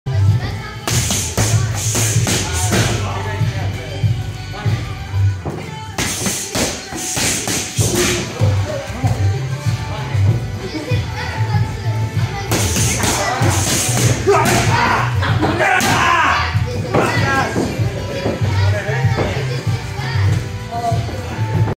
Stuart "The Insane" Fontaine putting in pad work with WSKB Green Belt Winston Bear
Coach Stuart "The Insane" Fontaine getting some rounds of pad work in with WSKB Green Belt Winston Bearboy of Makwa Sahgaiehcan First Nation.